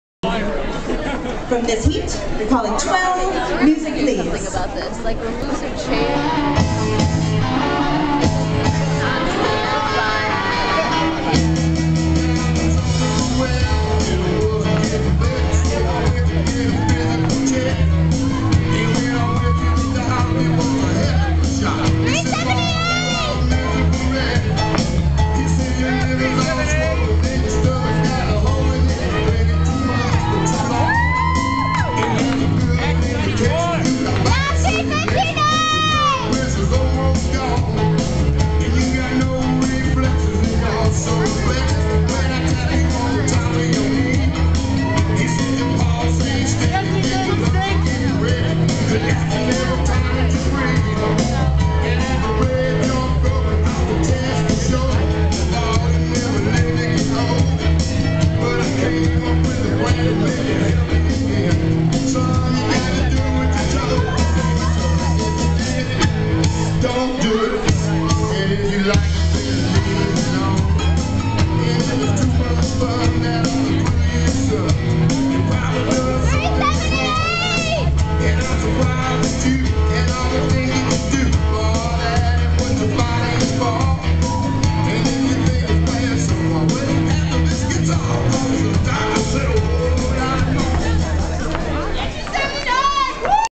009 Newbie Swing.MOV
2011 Brown Ballroom Competition
Moseley's on the Charles
Sunday November 13, 2011
Dedham, MA